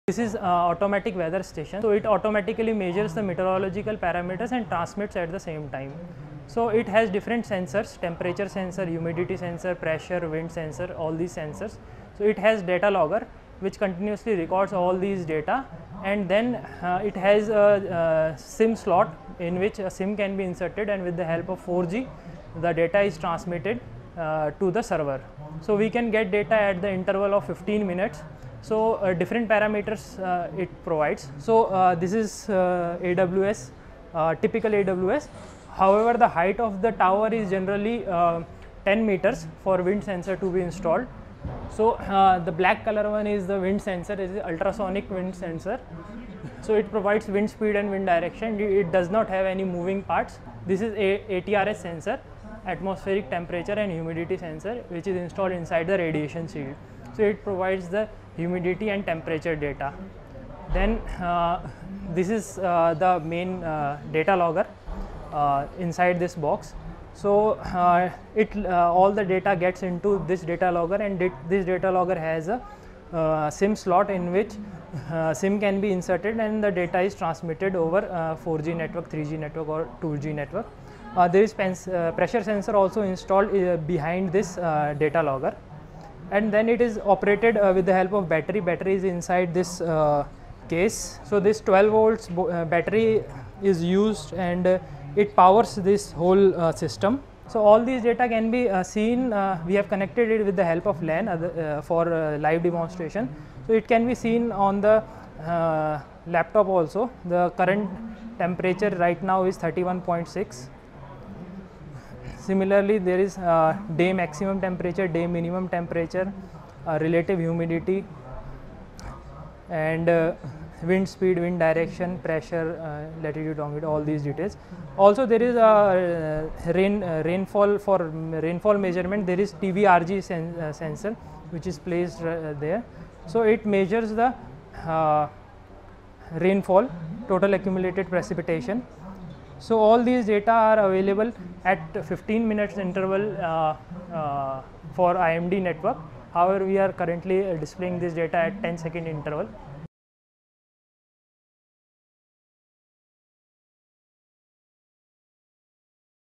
How Automatic Weather Station (AWS) provide data on weather parameters? | Ministry of Earth Sciences
Watch Mr Rohit Thapliyal, Sci C, IMD Dehradun, explains the functioning of an AWS.
How Automatic Weather Station (AWS) provide data on weather parameters such as temperature, rainfall, wind, and atmospheric pressure every 15 minutes?